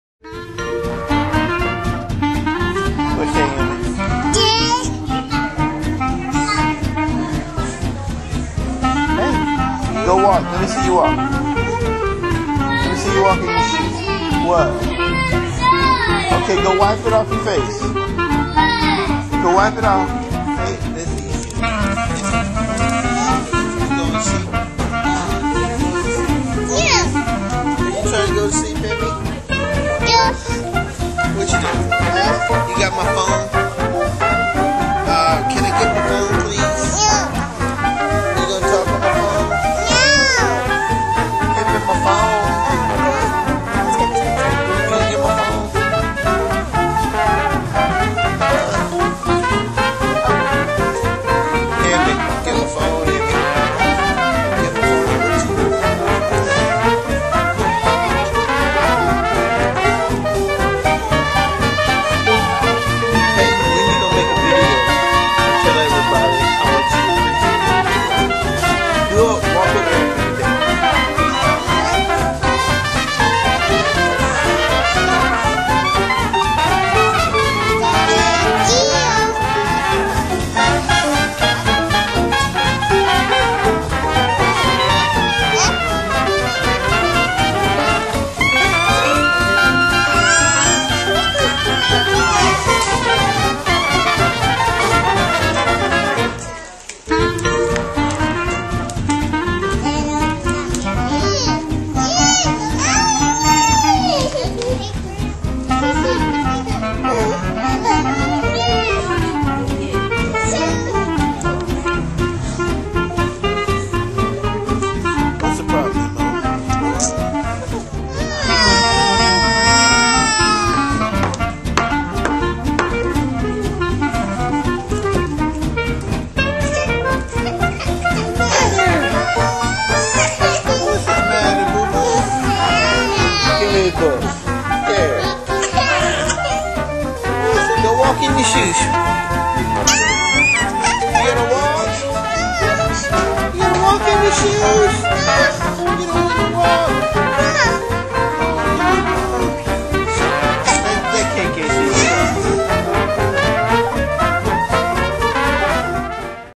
The Girls playing around.wmv
Trin-trin, Dori-G and Emi-Grace are all just playing around having fun